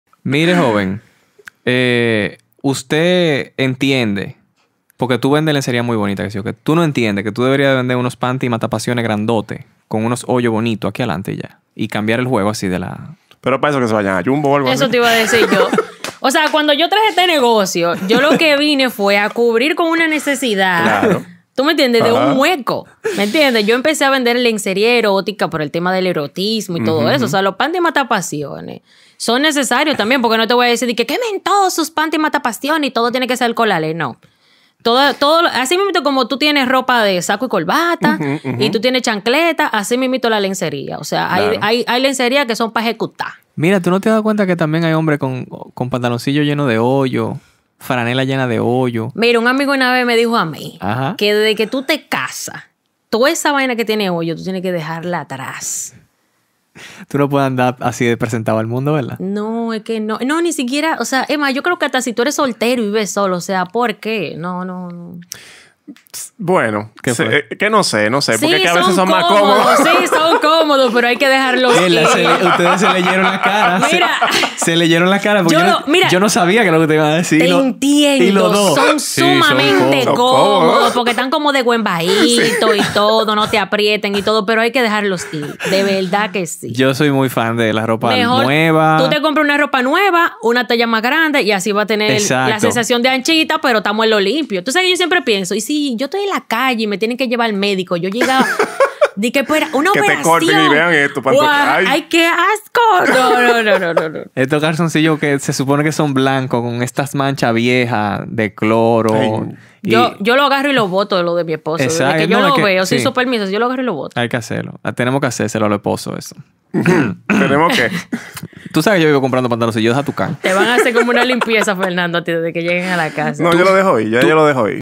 Los panties mata-pasiones
🎥 Equipos de Grabación y Recomendaciones

Volvemos a contar con la participación de Patricia Guzmán, hablamos de juguetes sexuales, el uso en pareja, los tabúes que aún existen en RD sobre este tema y si los hombres visitan sexshops y si que cosa son las que más suelen comprar las mujeres en la tienda.

👍 Dale a "Me gusta" y compártelo, me ayuda muchísimo y te lo agradeceré por siempre. :)